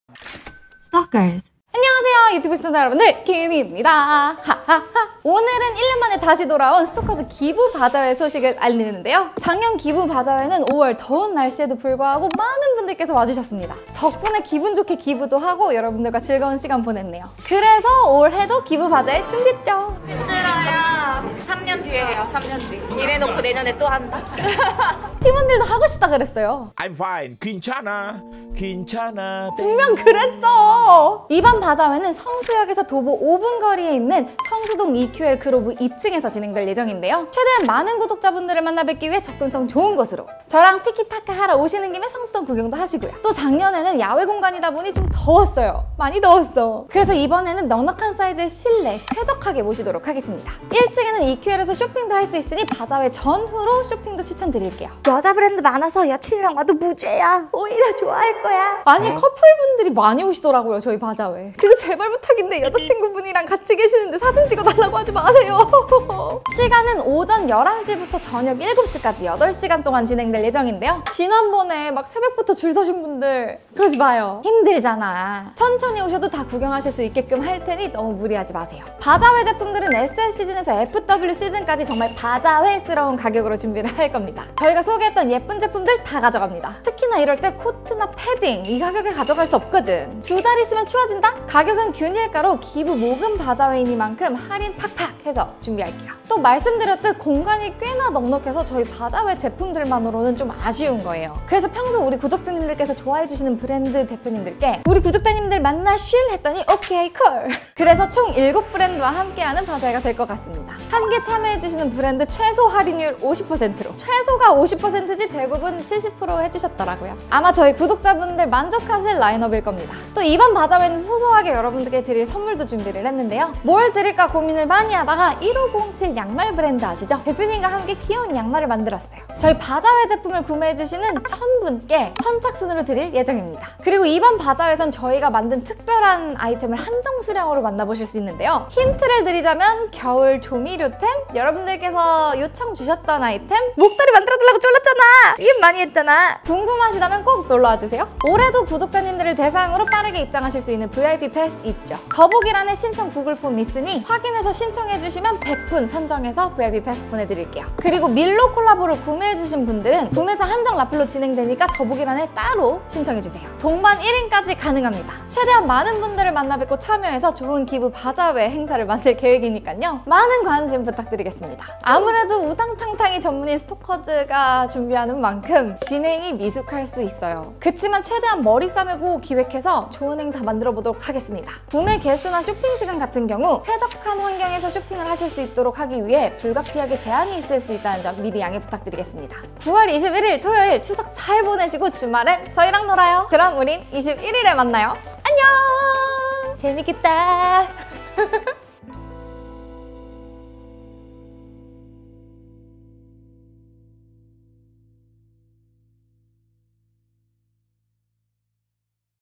역대급!! 스토커즈 바자회합니다!
✅오늘 영상에 키미가 입고 있는 제품은?
[스토커즈 x 유니온블루] EDITOR SHIRT INK NAVY 입니다!

안녕하세요 키미입니다~💚
1년 만에 다시 돌아온 스토커즈 바자회🥳
저랑 놀아주시러 오실 거죠?🥹🥹

🟠날짜 : 9월 21일
🟠시간 : 오전 11시 ~ 오후 7시
🟠장소 : 성수 EQL 2층 (서울 성동구 연무장15길 11)

🟢선착 1000분께 한정 특별 양말 증정!
🟢스토커즈의 비밀스런 아이템 선공개 및 할인 혜택!
🟢한 번에 바로 입장 가능한 VIP PASS 100분 선정!

[VIP PASS 신청은 9월 7일까지 입니다!]

🟡[밀로x스토커즈 콜라보 구매자] 전용 구글 폼

🌟PRODUCT INFO🌟